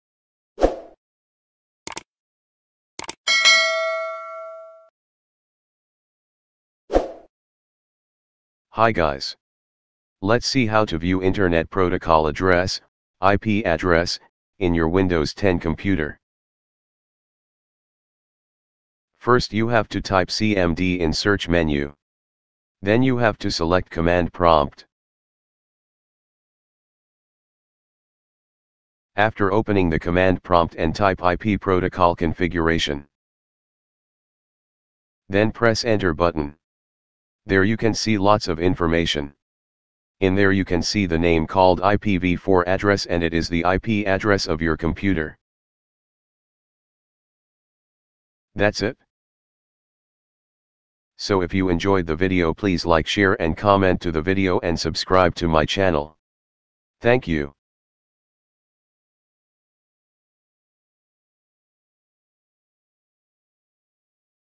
How to View IP Address in windows 10 computer using command prompt
the easiest way to  View IP Address on Windows 10 computer using cmd